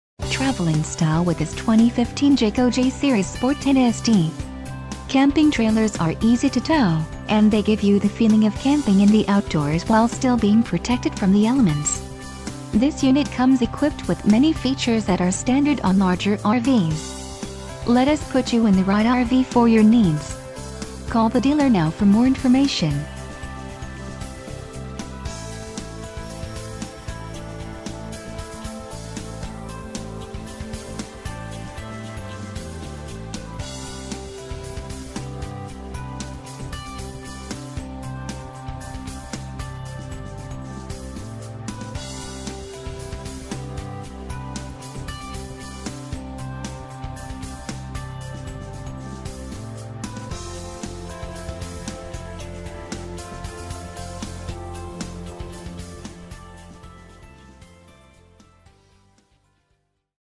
2015 Jayco Jay Series Sport 10SD - Fretz RV - Souderton, ...
For Sale in Souderton, PA 18964

2015 JAYCO JAY SERIES SPORT 10SD POP-UPWE CAN ONLY ADVERTISE MSRP, CALL FOR SALE PRICE Comes with Jayco&apos;s Customer Value Package, 2.0 cubic foot refrigerator, A/C prep, furnace, electric brakes, spare tire and canopy awning.FREE Camping membership with every purchase, click here to learn more.Like us on Facebook!Travel Height: 57&quot;, Length Overall (LOA): 162, Exterior Width: 85&quot;, Fresh Water Capacity (gals.) including WH: 10, Unloaded Vehicle Weight (lbs.): 1,435, Dry Hitch Weight (lbs.): 165, Gross Vehicle Weight Rating (lbs.): 2,250, Campsite Length: 19&apos; 6&quot;, Cargo Carrying Capacity (lbs.): 815, Exterior: 12&quot; D-range radial tires with polethylene wheel wells, 120V GFCI protected exterior receptacle, 12V electrical system with 110V converter, 20-lb. propane gas bottle, 30-amp power cord, Bumper-welded steel construction, City water hookup, Domed, seamless fiberglass roof, DuraTek water-repell... read more